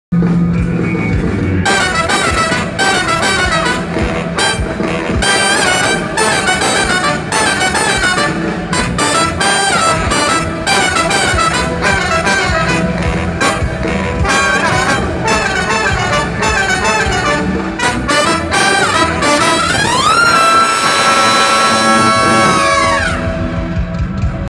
JG Brass section
María José.

Así probaba el audio la sección de metales de Juan Gabriel, esta fue la alineación que estuvo con él en sus últimos conciertos

Bernardo Castillo (TP 1)
Isaías Jimenez (TP 2)
Manuel Hernández (TB 1)
Enrique Sida (TB 2)
Ángel Ramos (SX 1)
Itzam Pacheco (SX 2)